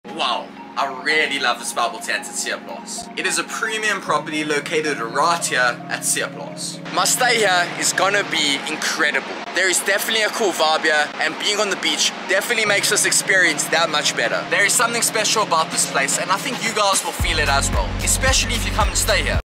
Bubble Tent on the Beach in South Africa
I stayed in the Premium Bubble Tent at Seeplaas in Groobrak, South Africa.
This place is awesome and I really enjoyed my stay here!
You have to come see it for yourself!

GEAR:

Sony zv1 (main camera)
What I use to edit
My Computer

If you’re reading this, I appreciate you more than you know!